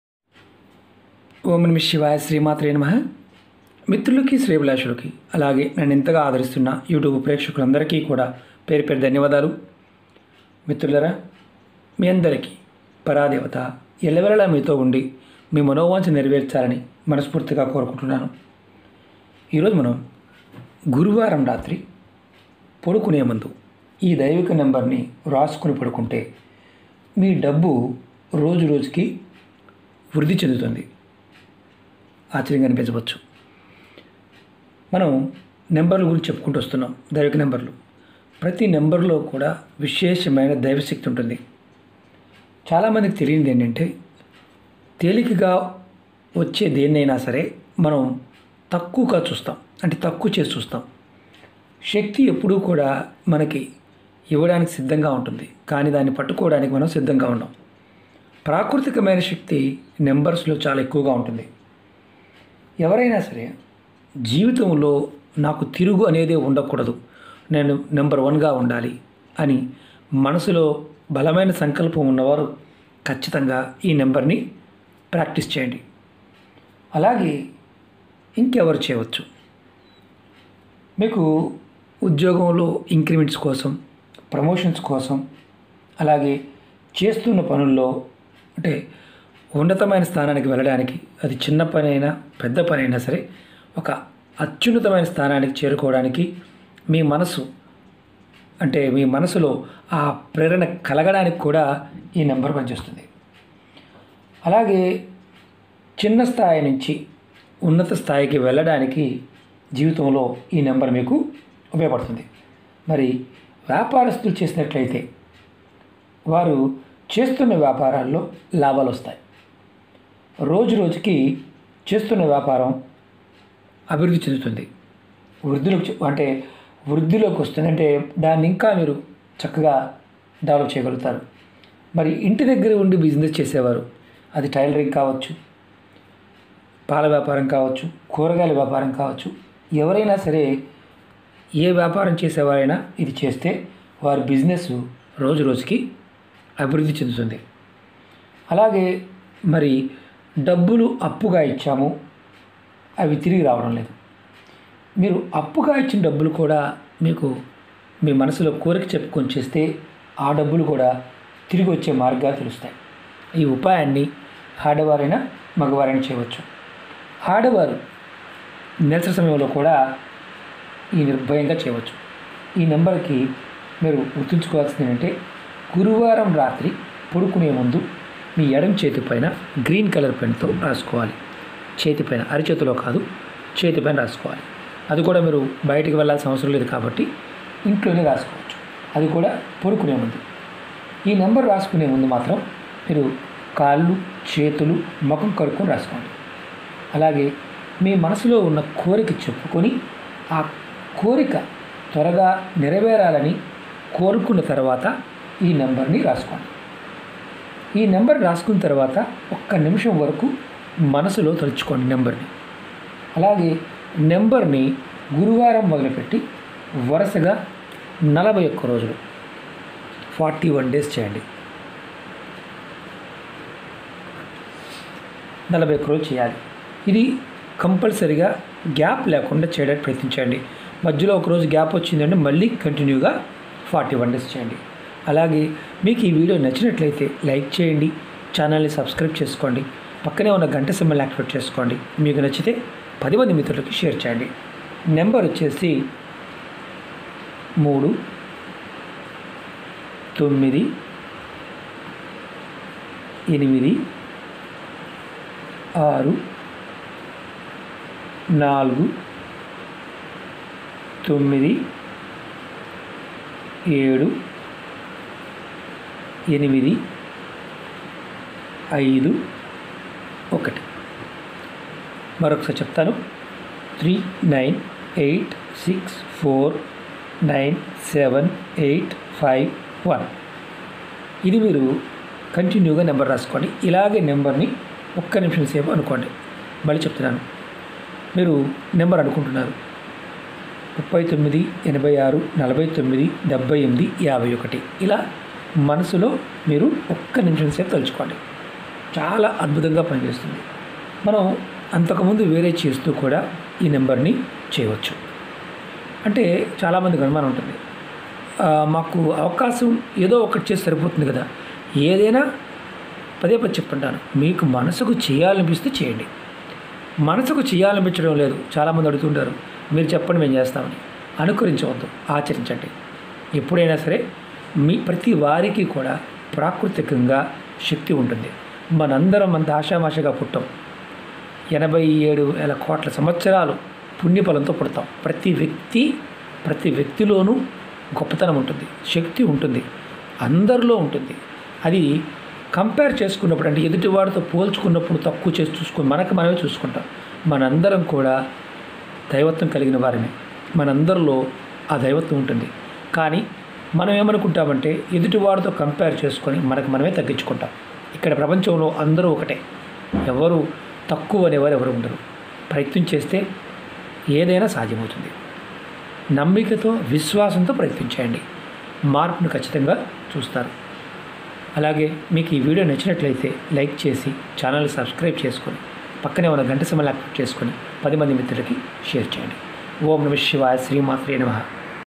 మీ డబ్బు రోజురోజుకీ వృద్ధి చెందాలి అంటే గురువారం రాత్రి పడుకునే ముందు ఈ దైవిక నెంబర్ రాసుకోండి
Time:10.00 AM TO 1.00 PM And 3.00 PM TO 6.00 PM
fees: 500 rupees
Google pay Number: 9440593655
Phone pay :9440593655